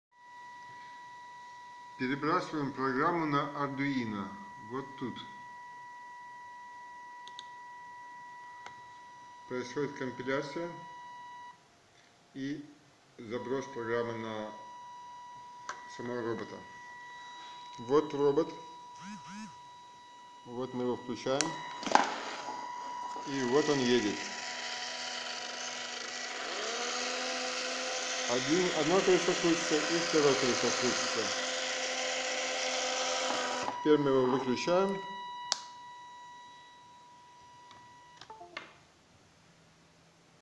2 motor control on motor-shield L293D
Управление 2-я моторами, на шилде L293D. Код внизу.